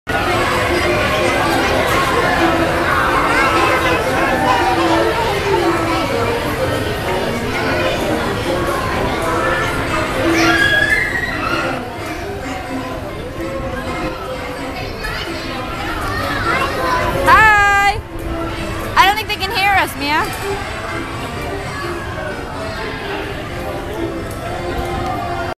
Mad Hatter's Tea Party
Ed and Alex escaped to enjoy some spinning fun on the Mad Hatter's spinning tea cups! The line was so short that morning we jumped right on after them and they joined us again!